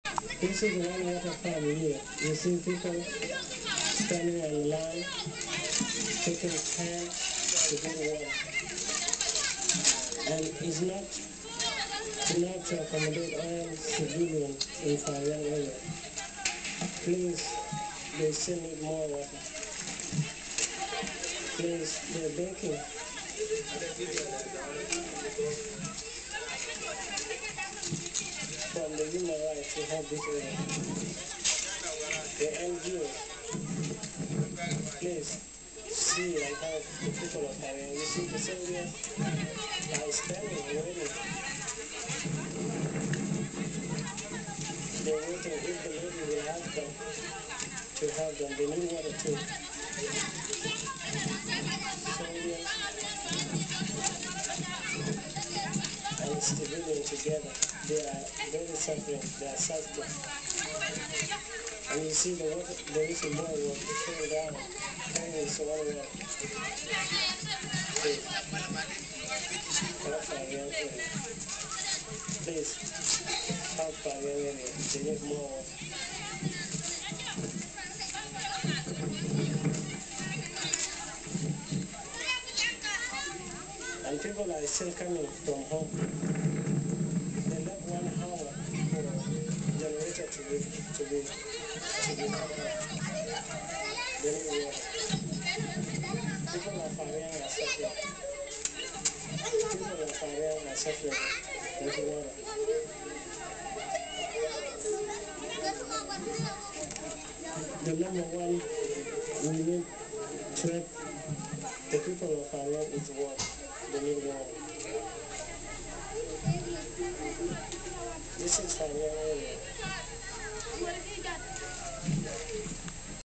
These ladies were fetching water at the point in Pariang 2006.AVI
This video was taken on the water point in Pariang Town by Ngorkur 2006.